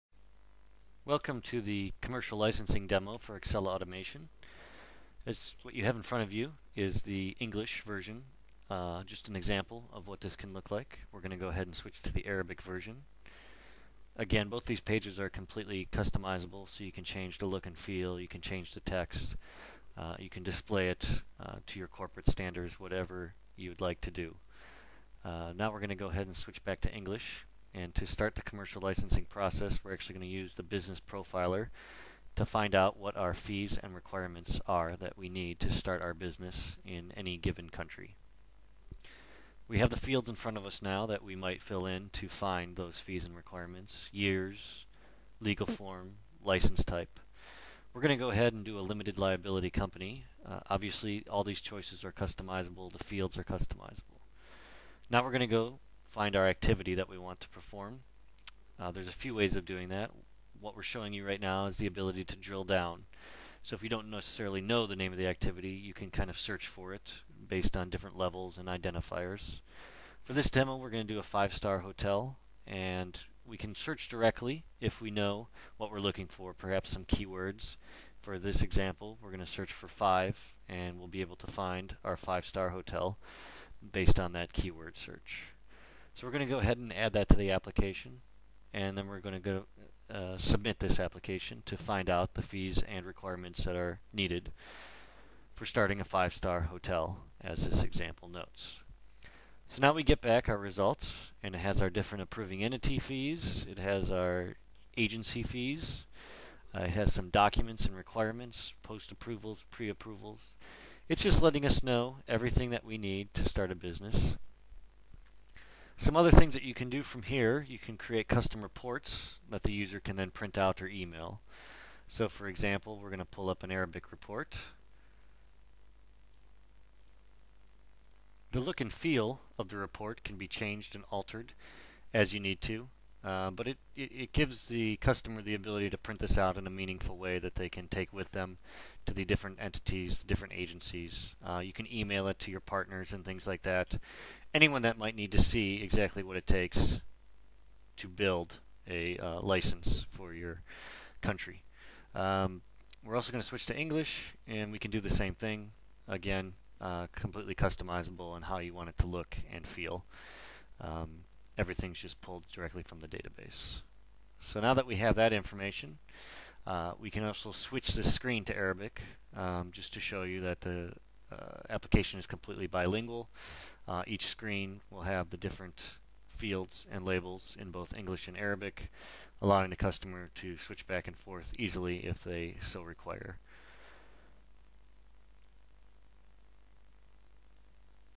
Demo of Multilingual e-Licensing in English and Arabic with Accela Automation
This 3-minute video provides a walk-through of the benefits of e-licensing using Accela Automation software's built-in multilingual capabilities, with seamless transition between English and Arabic.